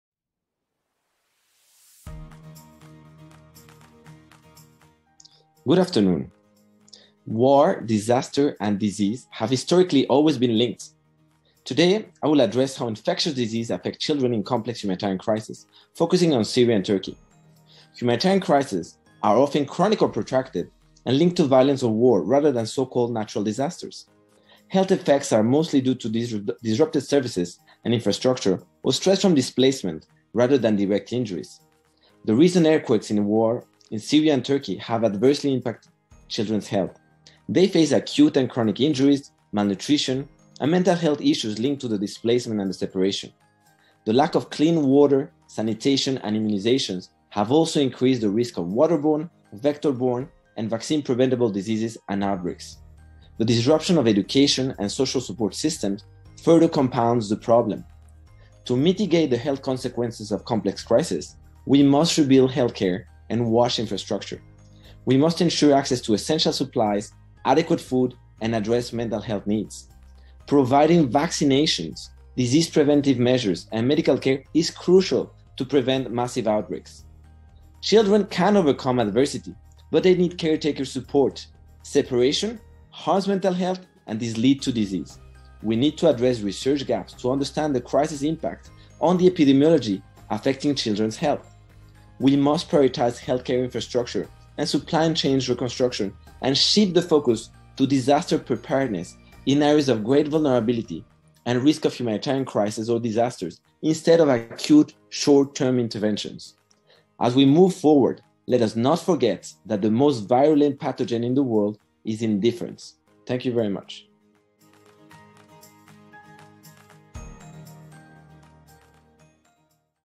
Daniel Martinez Garcia - The Consequences of Syria and Turkey Earthquakes and War for Child Health
ESPID 2023 Speaker - Daniel Martinez Garcia - The Consequences of Syria and Turkey Earthquakes and War for Child Health

Join us in Lisbon & Online